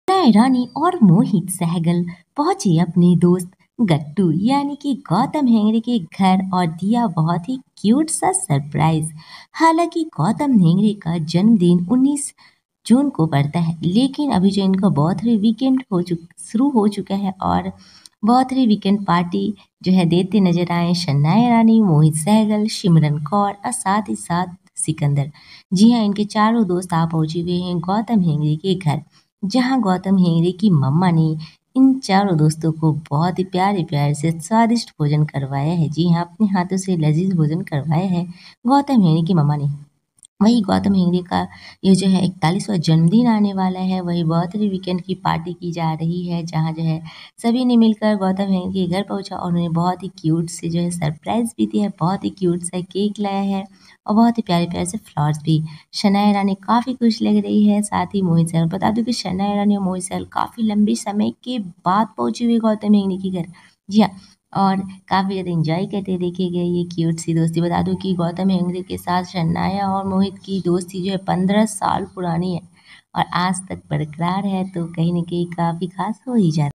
Sanaya Irani Mohit Sehgal Kaur gave surprise to gautam Hegde 41st Birthday Weekend
Sanaya Irani Mohit Sehgal simran Kaur arrived gautam Hegde home and gave surprise 41st birthday party